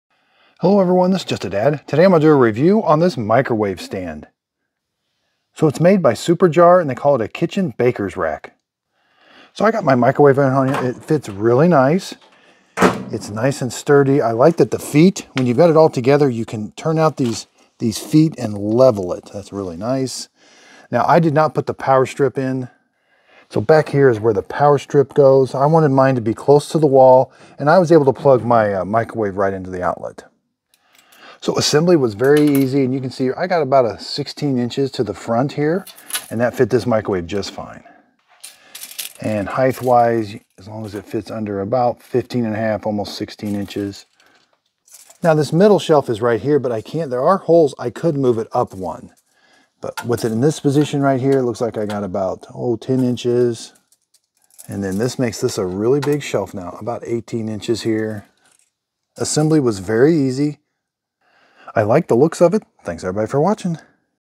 SUPERJARE Kitchen Bakers Rack Review. Great as a Microwave Stand, Coffee Bar, Storage
SUPERJARE Kitchen Bakers Rack.  Great as a Microwave Stand, Coffee Bar, Storage.  Comes with a power outlet.  Easy assembly